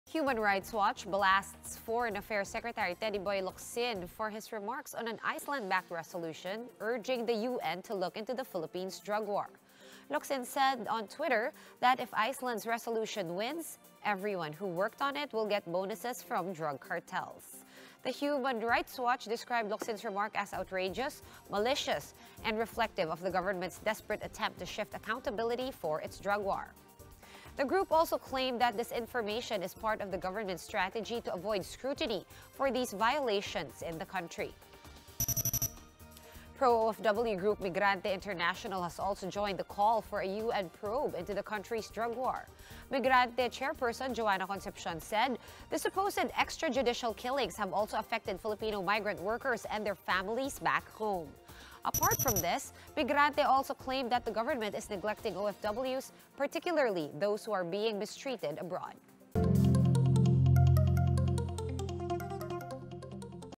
HRW blasts Locsin over remark vs. Iceland resolution
Human Rights Watch blasts Foreign Affairs Sec. Teodoro Locsin Jr. for his remarks on an Iceland-backed resolution which urges the United Nations to probe Duterte's drug war.